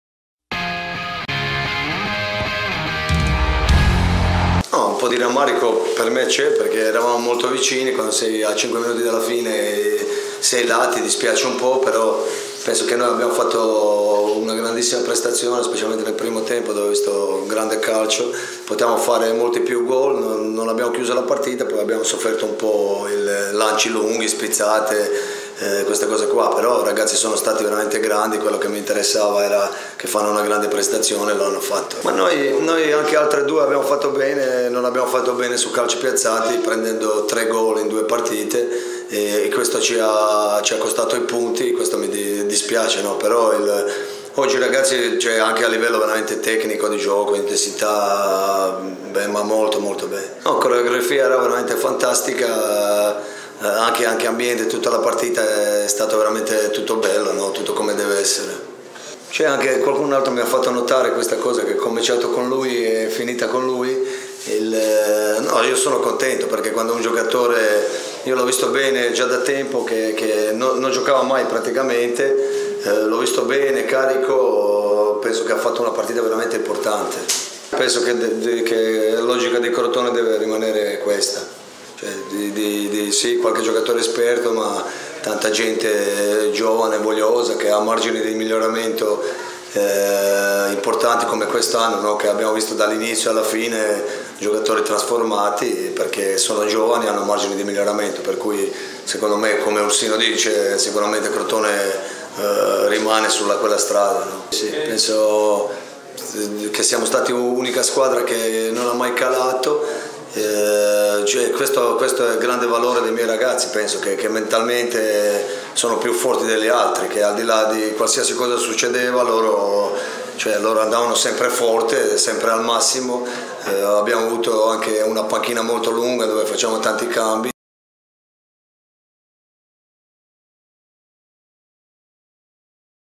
Post Entella, Juric in conferenza
Il video della conferenza stampa al termine dell sfida tra Crotone e Entella. Ai microfoni di FcTv parla il mister pitagorico Ivan Juric.